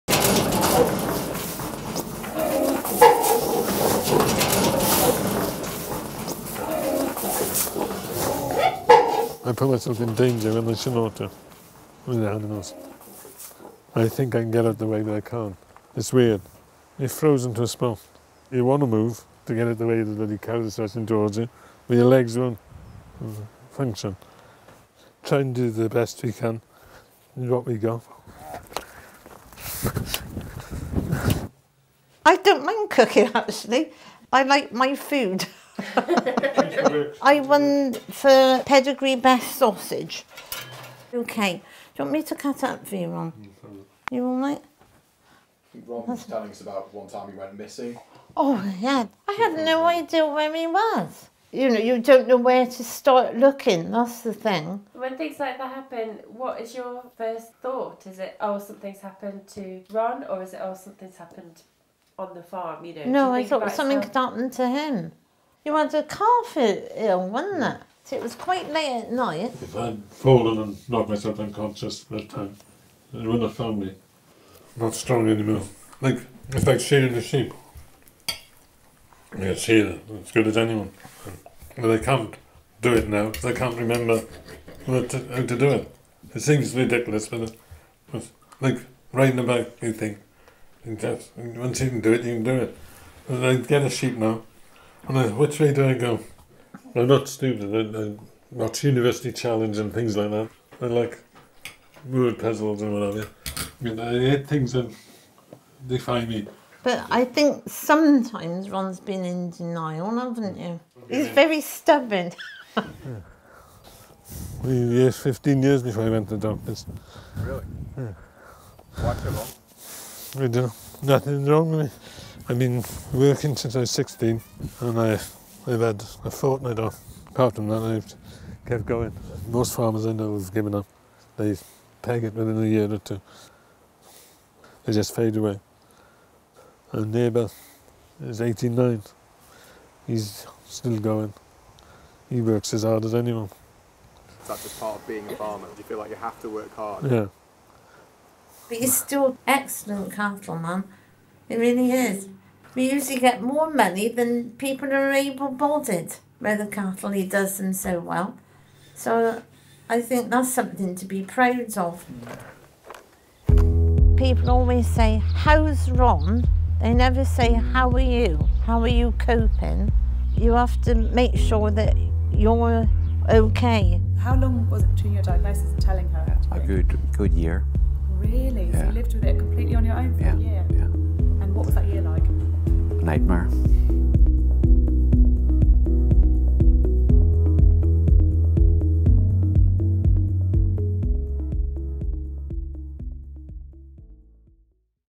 Parkinson's freezing
Freezing and memory loss sometimes get in the way when Ron is working on his farm. Do you ever experience freezing? What helps you get moving again?